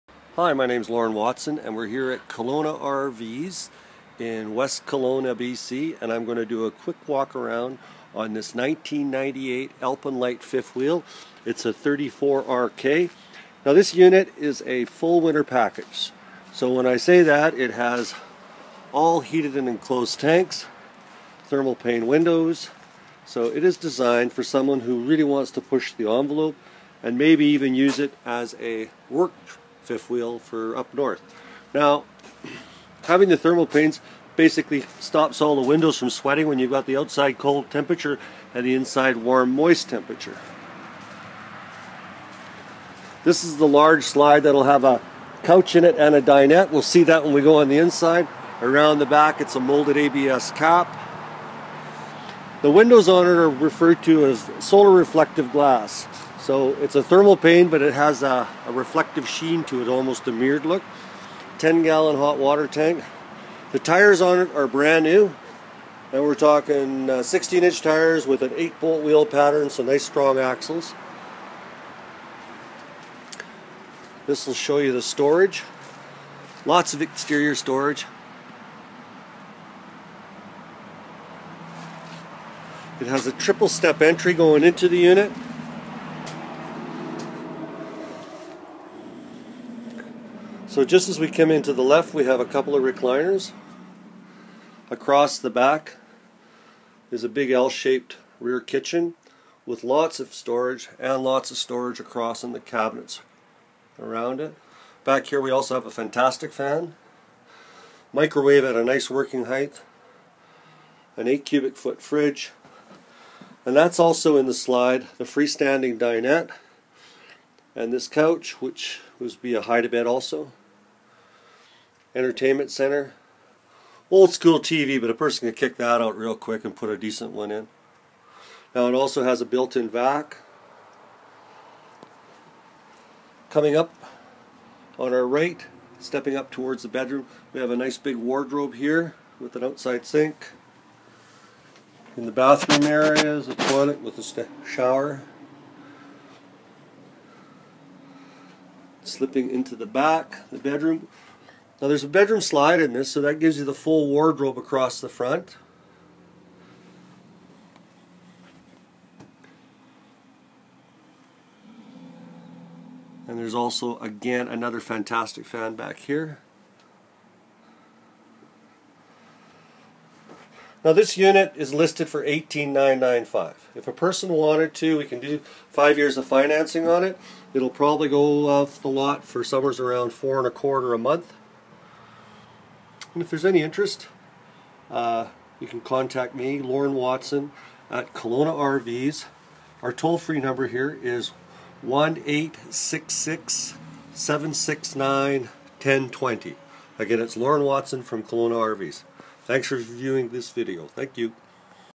1998 Alpine Lite 34RK- Limited Edition
5th wheel with a full winter package it's a true four season unit.